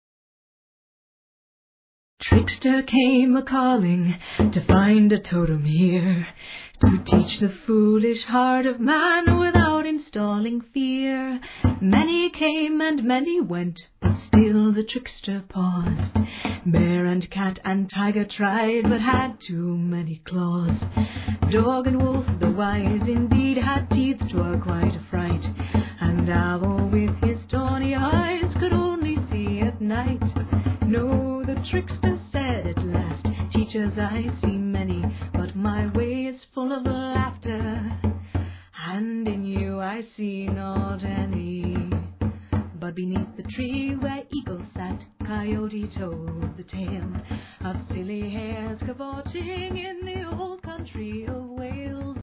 Rabbit's Song Picture Book Trailer
Trickster comes a-calling
To find a totem here—

In the time before time, Trickster searches among all the animals for a teacher of man. One by one, he examines and dismisses Bear, Cat, Tiger, Dog and Wolf as being too frightening. Trickster at last settles on the humble and unimposing Rabbit with his companions, Coyote, Raven and Crow as the very beasts to teach the Way of Laughter.

As the story unfolds, the reader is taken on a journey of warmth and humor full of visual delights and details reminiscent of the worldwide Trickster tales which sparked the story's inspiration.